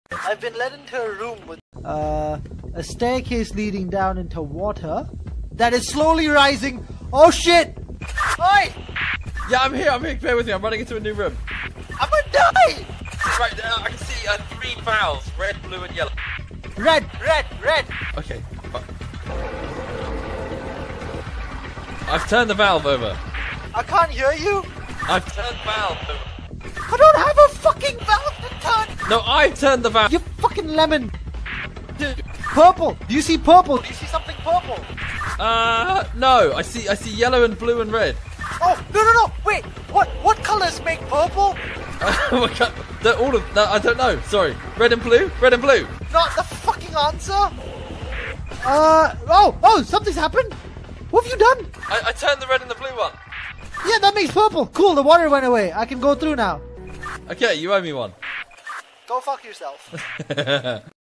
Teamwork animatic gift
this is a gift for demon c he is a friend of mine and the the fox the second character is me in animal form